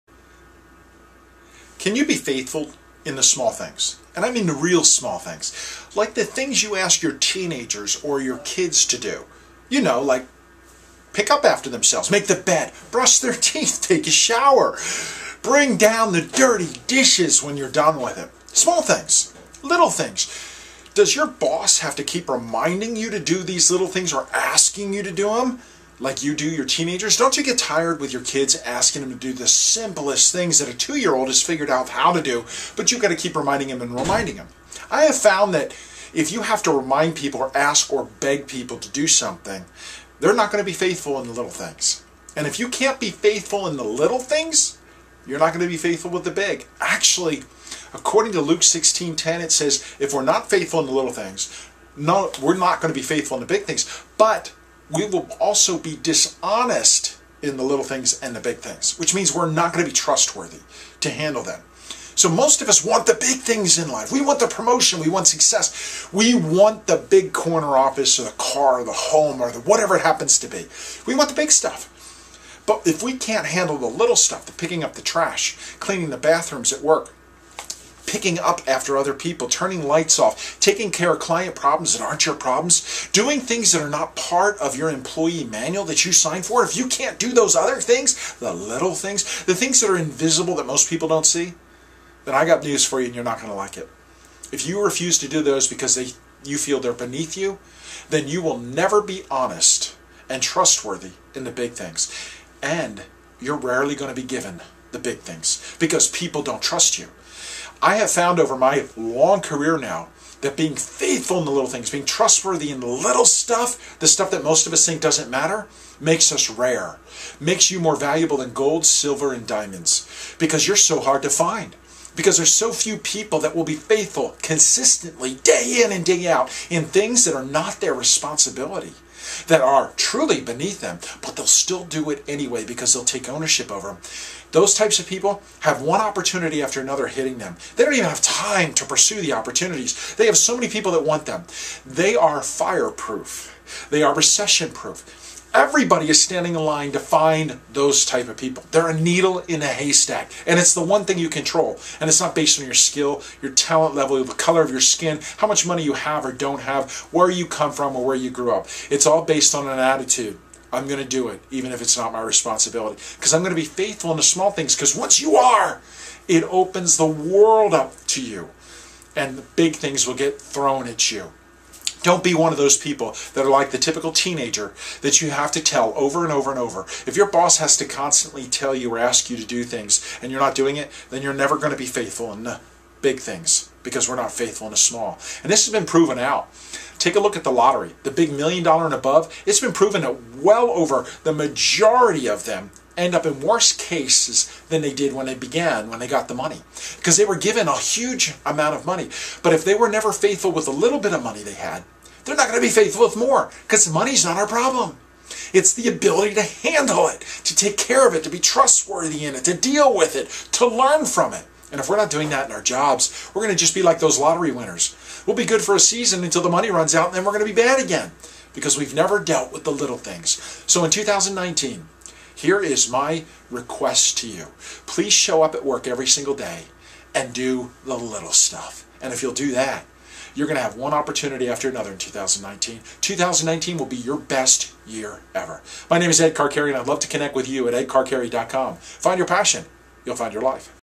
[Pod Cast] Ed Carcarey/Can You Be Trusted
Can you do the little things at your job without being asked or told? Things that you think are beneath you? Are you Faithful with the small stuff? If so you are more valuable than Gold!